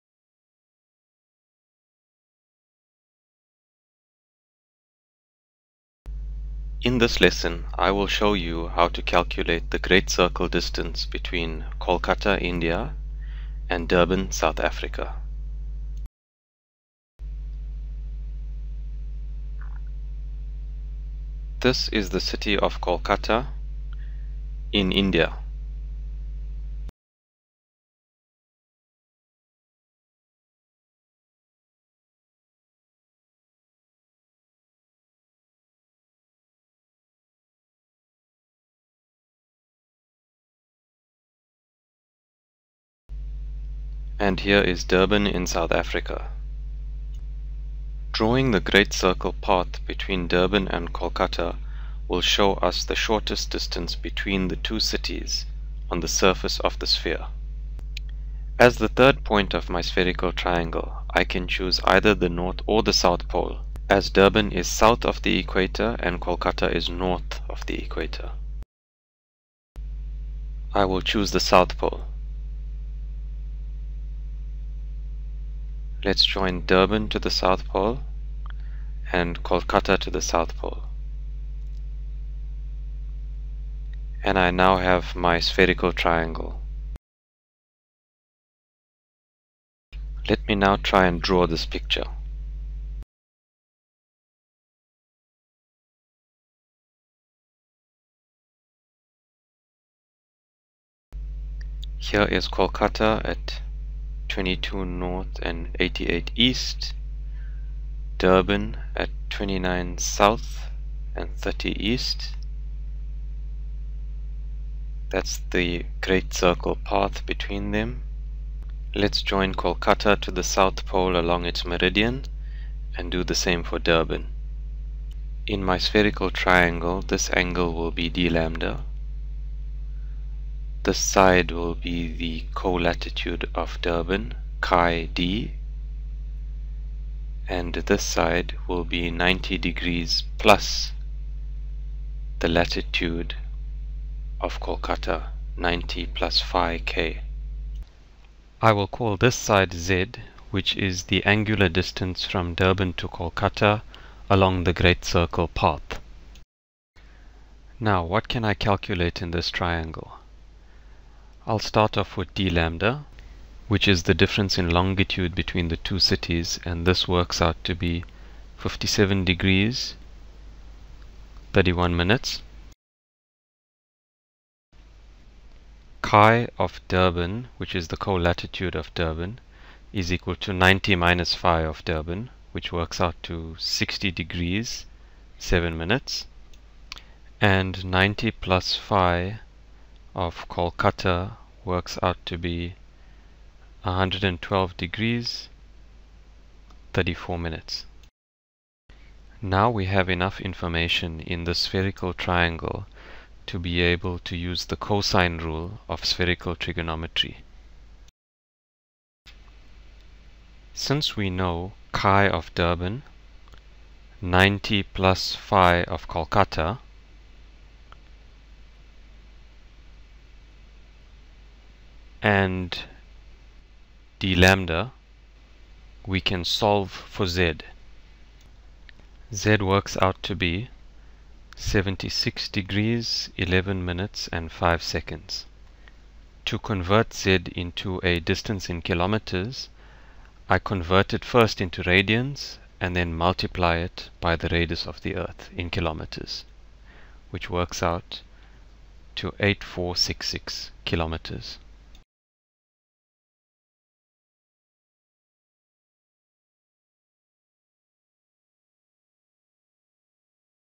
Spherical Trigonometry - Durban-Kolkata
A calculation of great circle distance between Durban and Kolkata